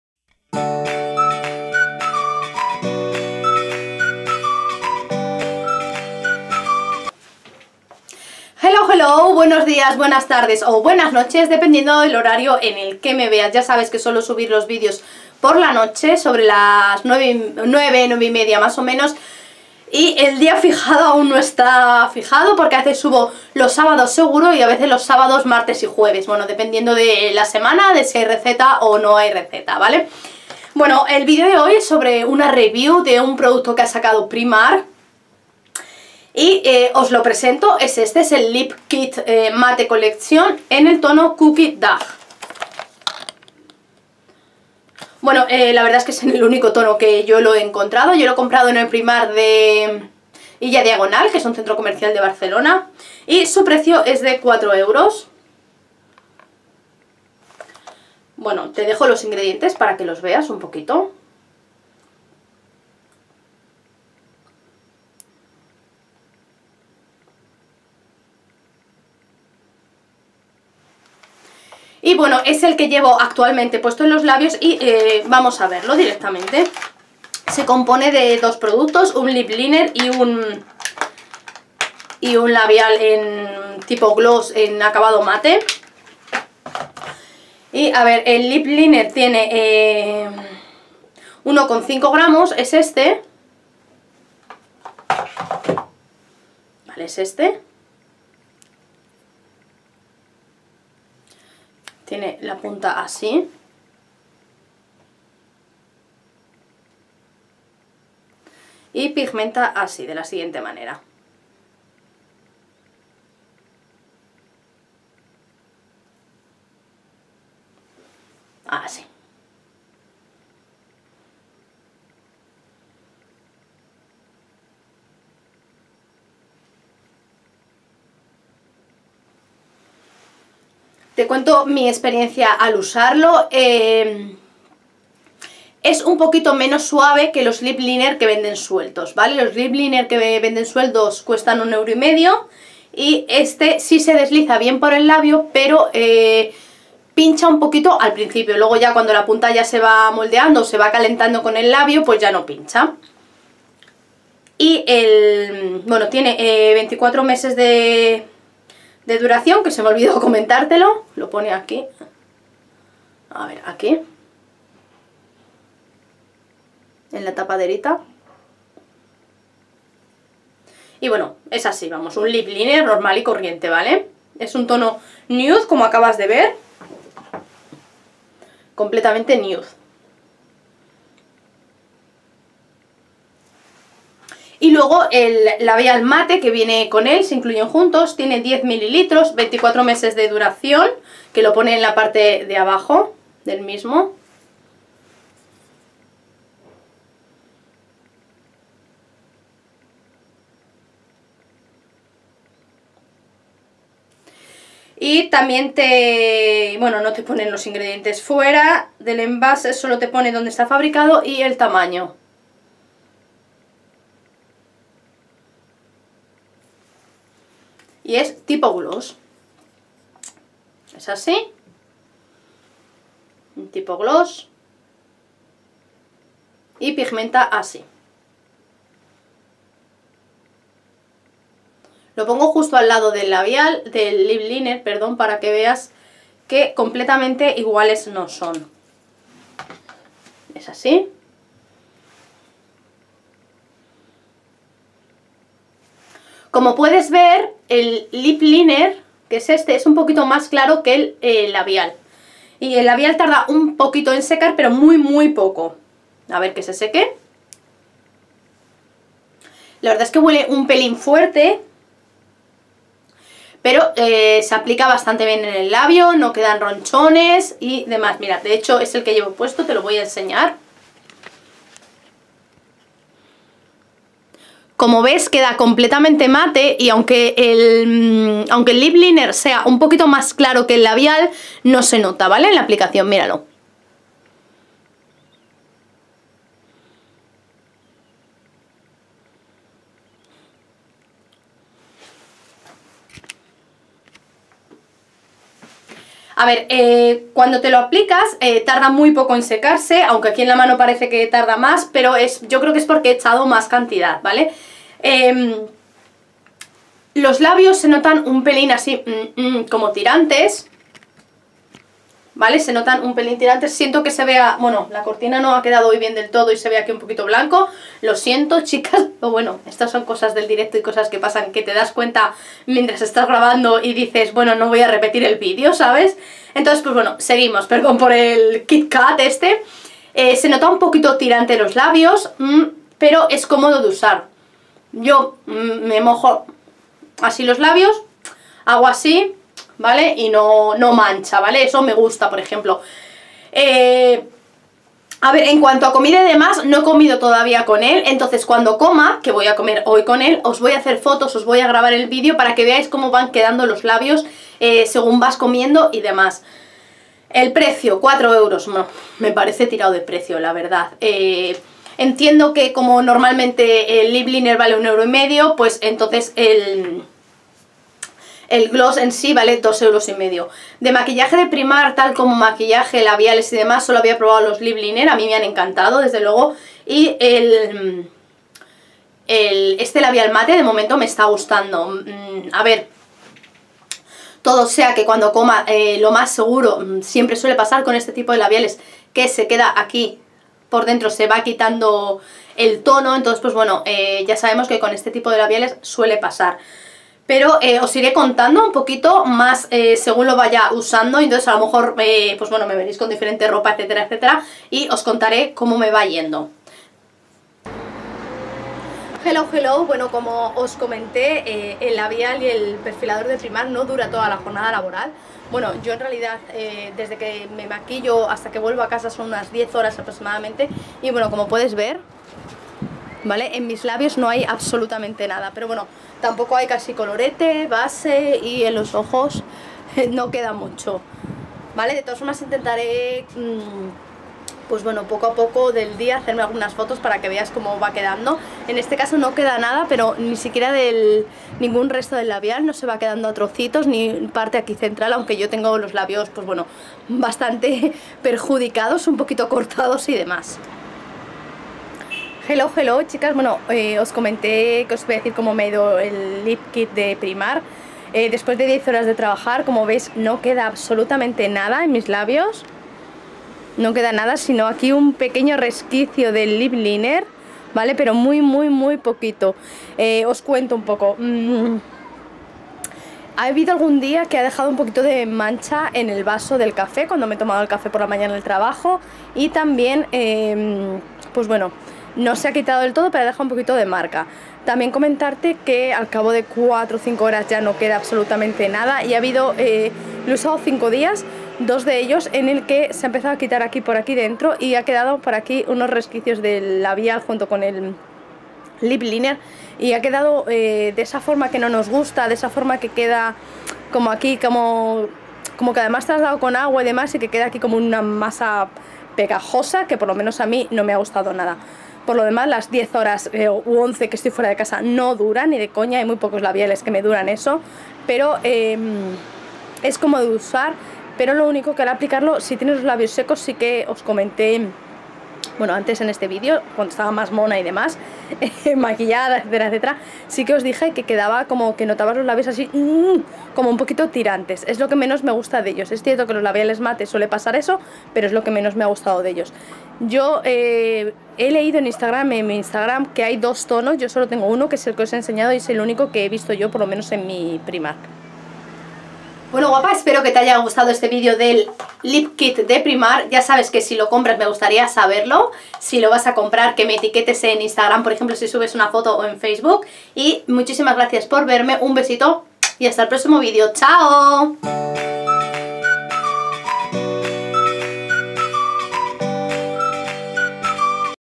PRIMARK | Lip Kit Matte Collection - Lip Liner + Matte Lipstick
Nuevos sets de Lip Liner + Matte Lipstick de Primark. Los encontrarás por 4 € en su sección Beauty y en dos tonos diferentes: Chocolate Brownie o Cookie Dough. Veremos este último, que es el más claro de los dos.

DESPLIEGA PARA ENTERARTE DE TODO !